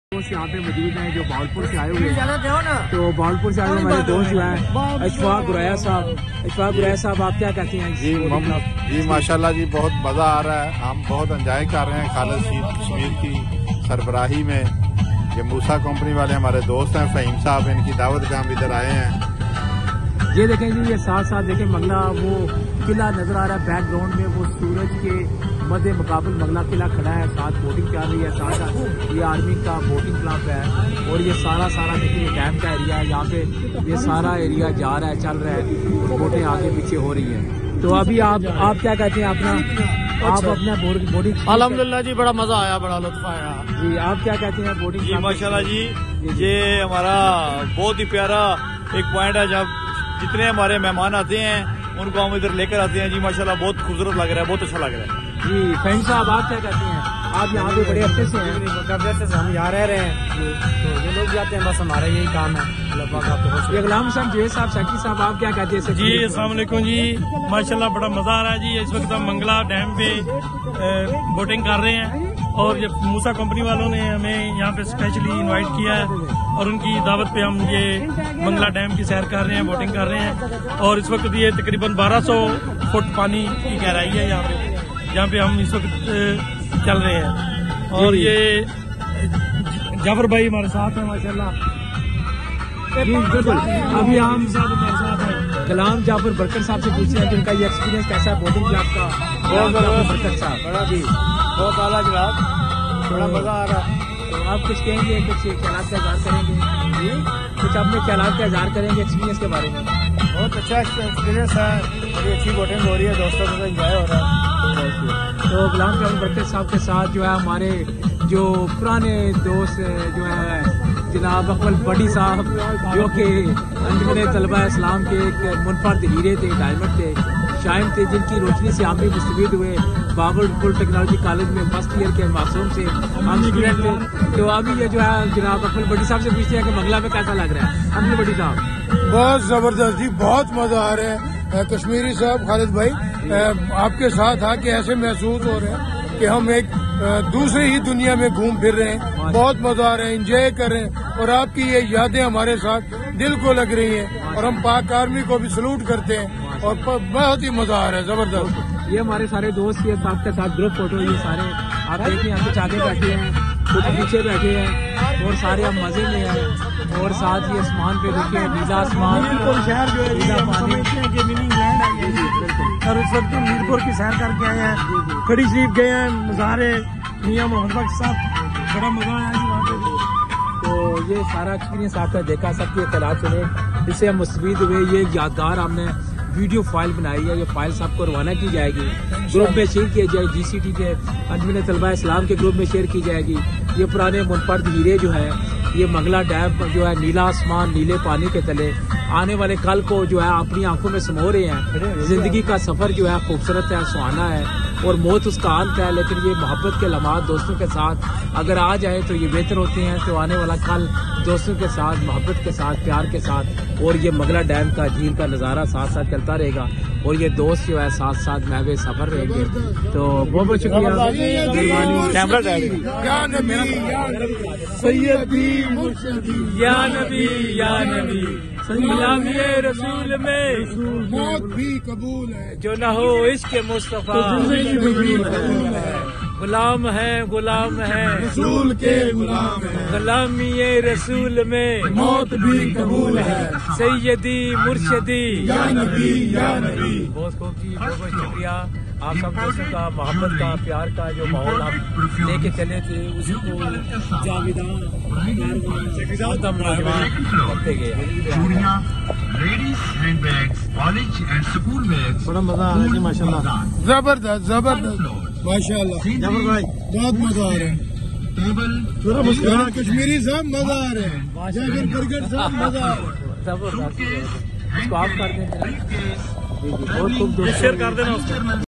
محمد اکمل بھٹی امیدوار قومی اسمبلی بہاولپور اپنے دوستوں کے ہمراہ منگلا ڈیم کی سیروتفریح کرتے ہوئے۔
محمد اکمل بھٹی امیدوار قومی اسمبلی بہاولپور و سابق رہنما انجمن طلباء اسلام اپنے دوستوں کے ہمراہ منگلا ڈیم  کی سیروتفریح کرتے ہوئے۔